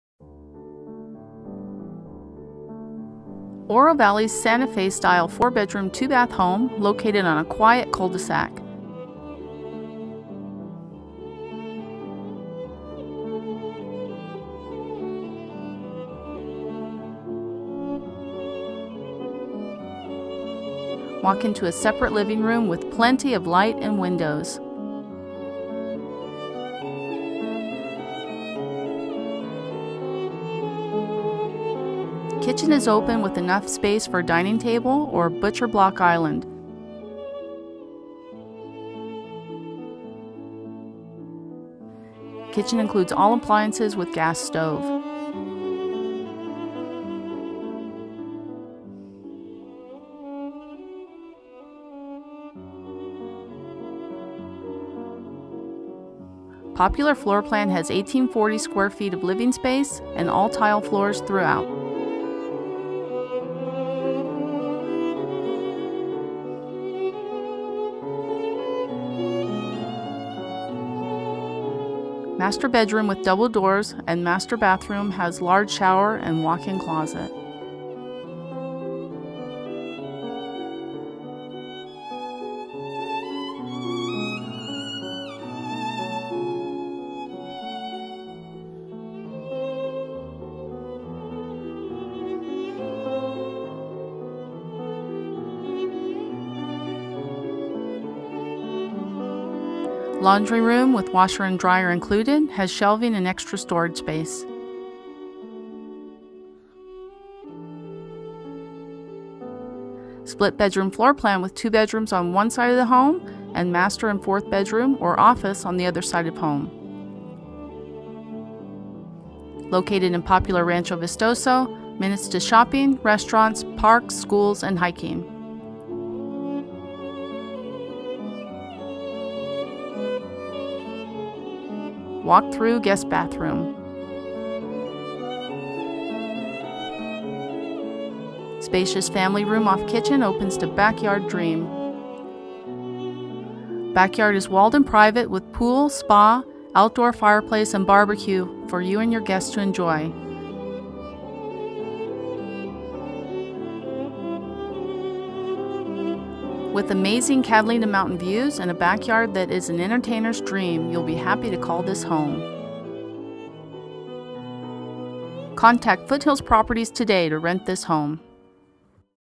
76 E Yucca Cove Oro Valley, AZ 85737
One of the best view lots in neighborhood, fantastic mountain views from rear patio pool - spa area, fenced pool, built in BBQ & outdoor fireplace. Separate living room and family room/ kitchen combo. Cul de sac location, close to grocery, hiking and sunsets. Ask about Property #2006.